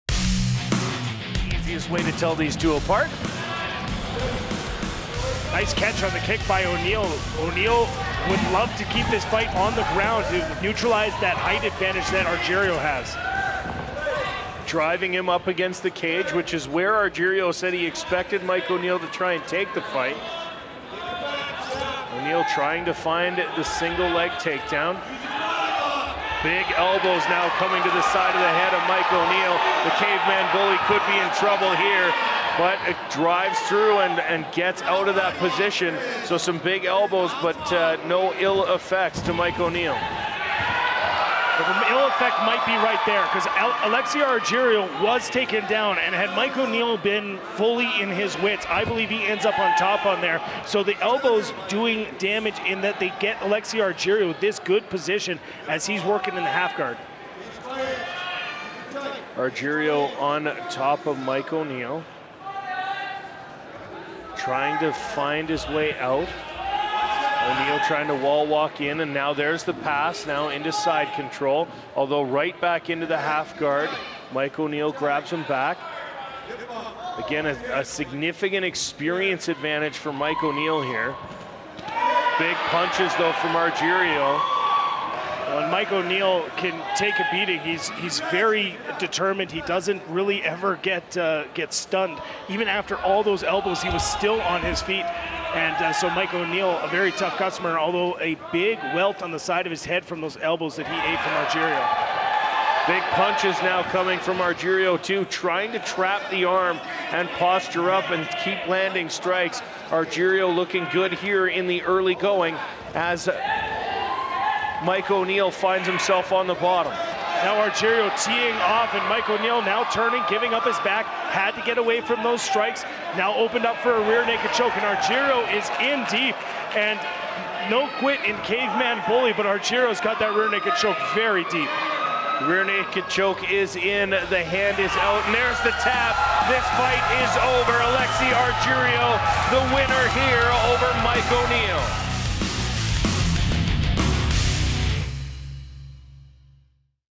Alexi Argyriou vs Mike O'Neill | MMA | Hard Knocks Fighting | HKFC 46
Professional and Amateur fighters from across North America battle it out in Hard Knocks' cage. Alexi Argyriou v. Mike O'Neill